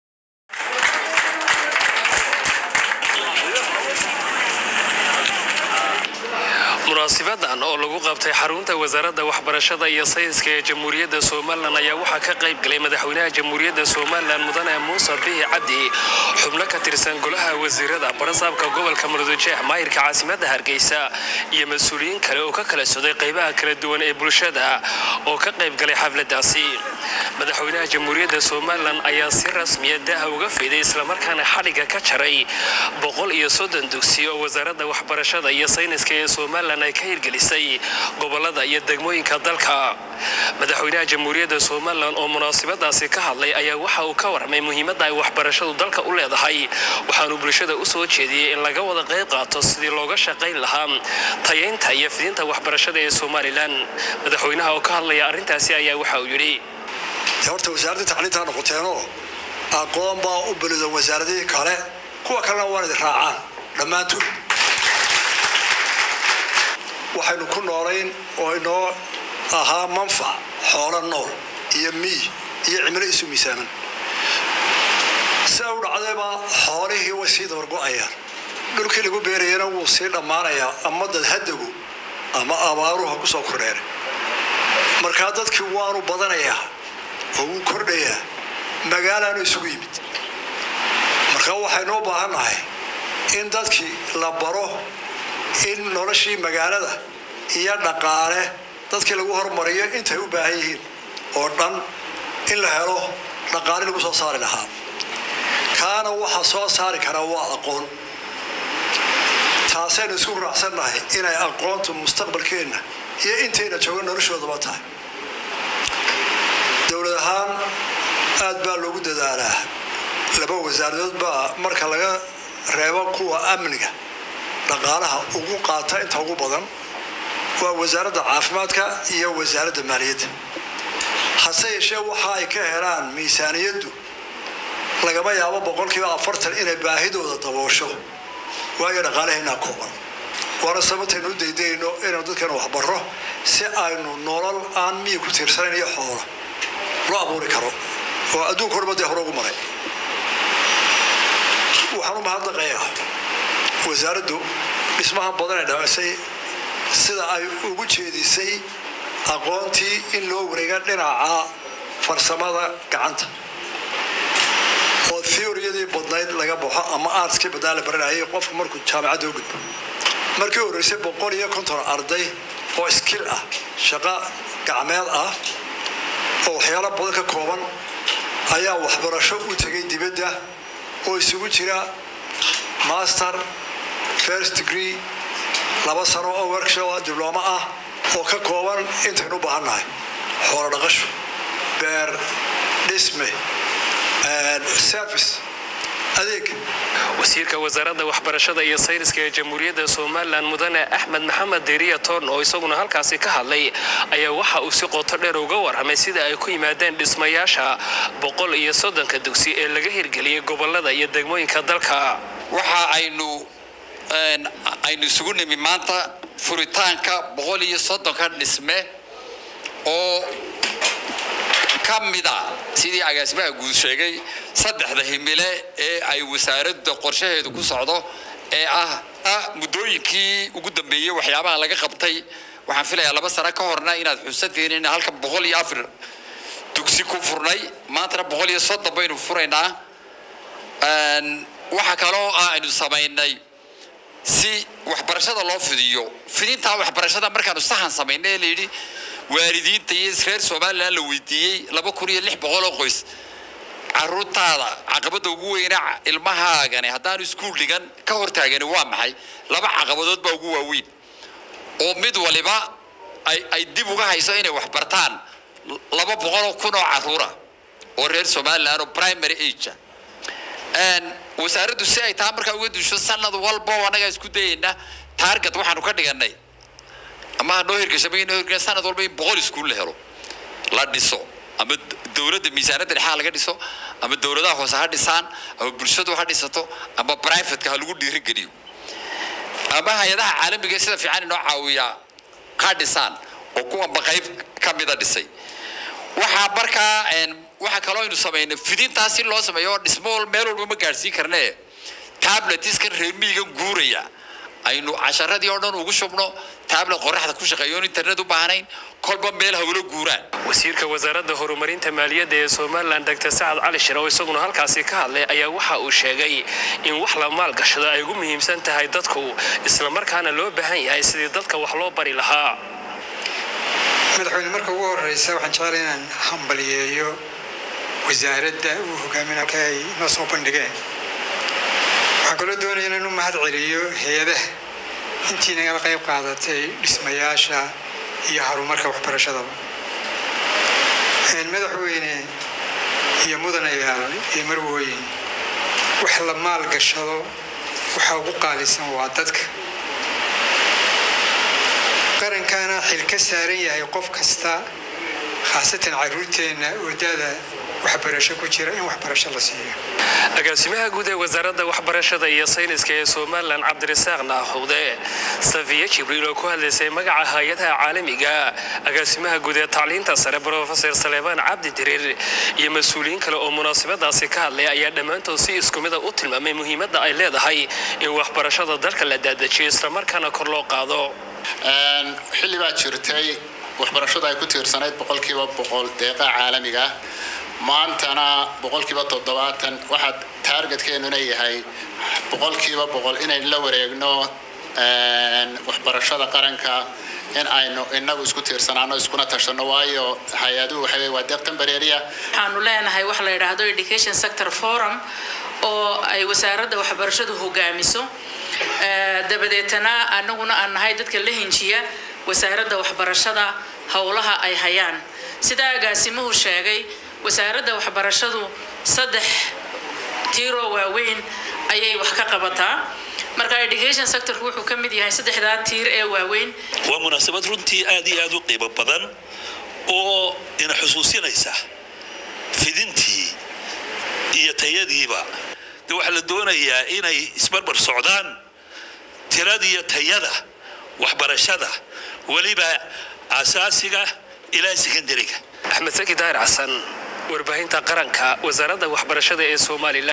Madexwenaha JSL MBC aya Xadhiga ka Jaray dhismayasha Dugsiyio Cusub Wasaradda Waxbarasho Hirgalisay
#130 New Schools # Waxbarashada JSL #